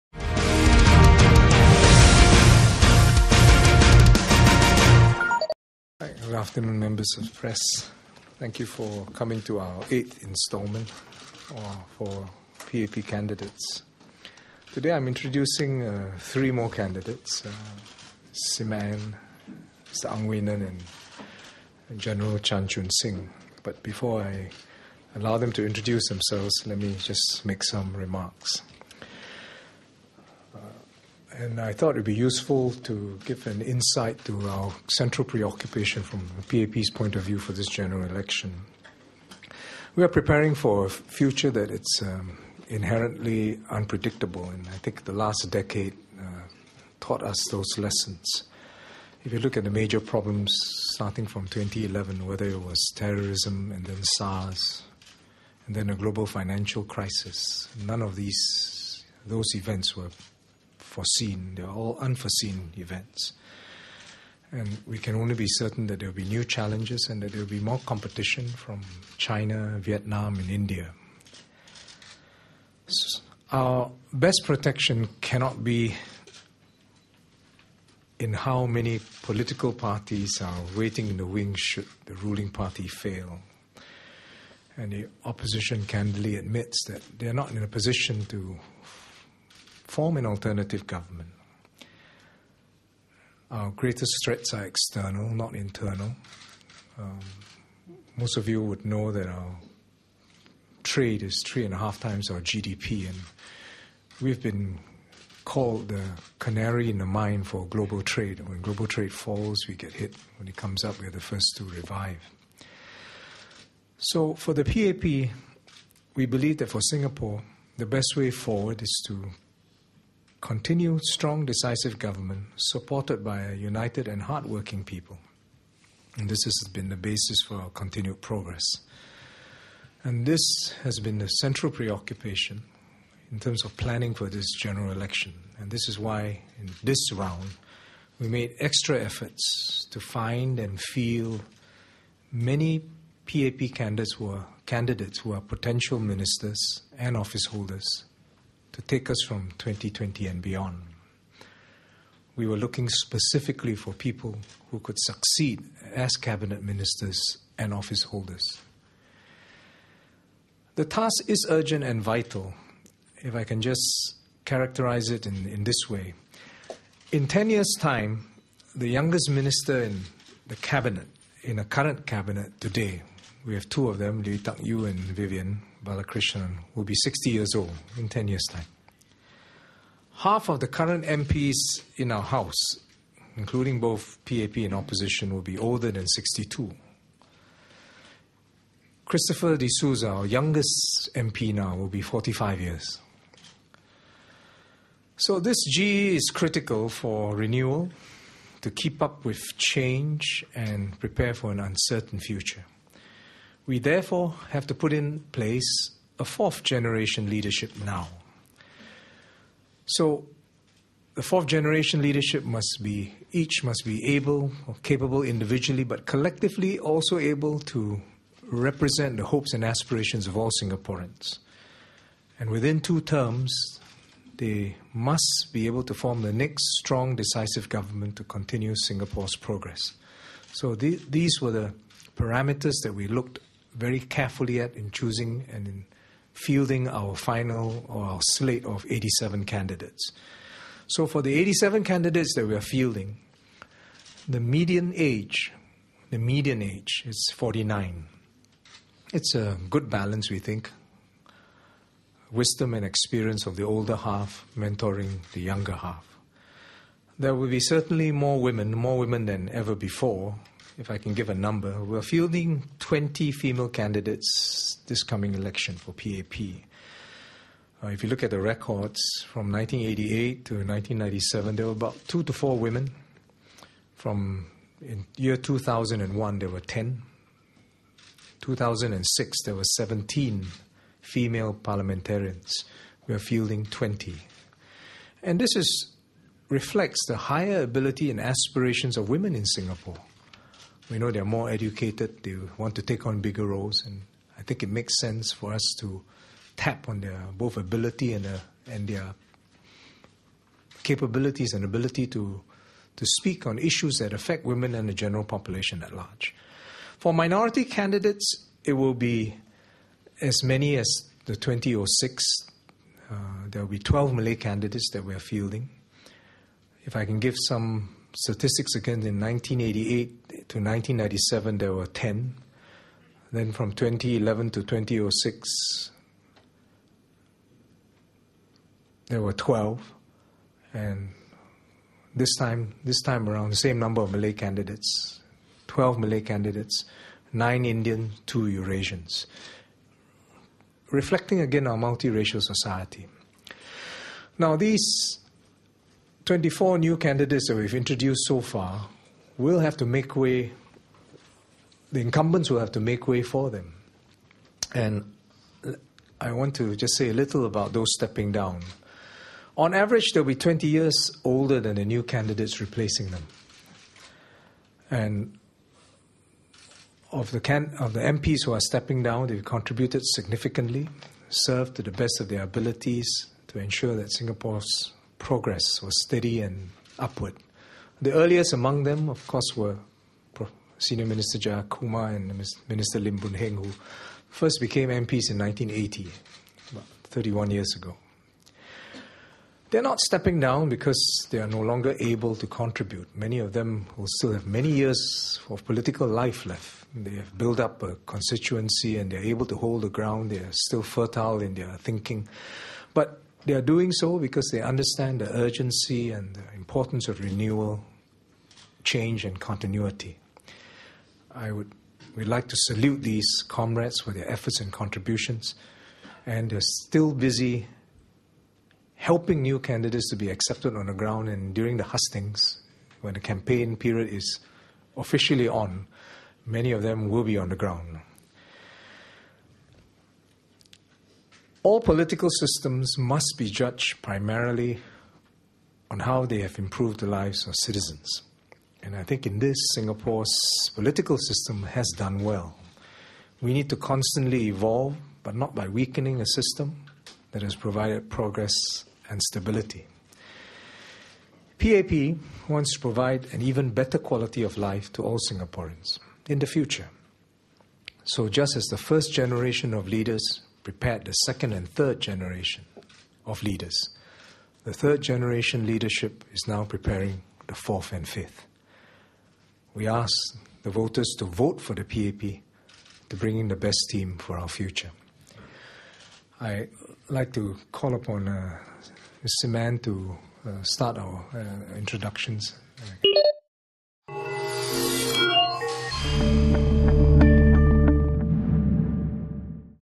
PAP introduces final batch of new candidates
Education Minister Dr Ng Eng Hen introduced the 8th batch of candidates on Thursday (April 14) at the party HQ.